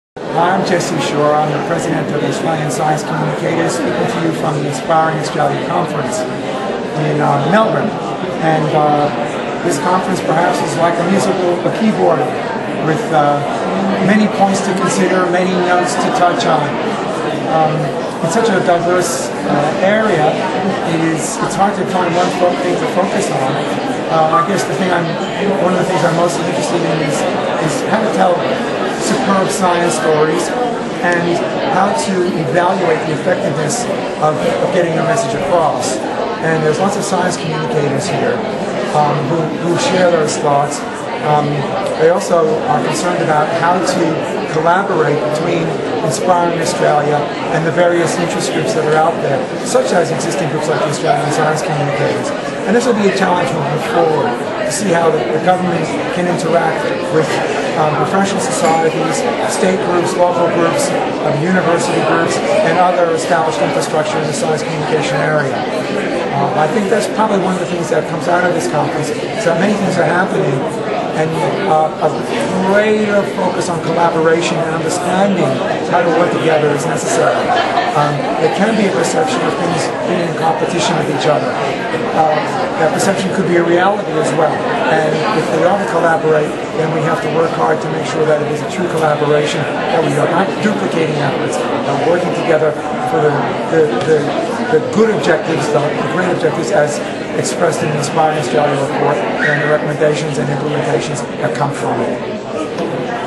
Jesse Shore, National President, Australian Science Communicators
Jesse Shore, National President of the Australian Science Communicators discuses the opportunities and challenges of the Inspiring Australia Report.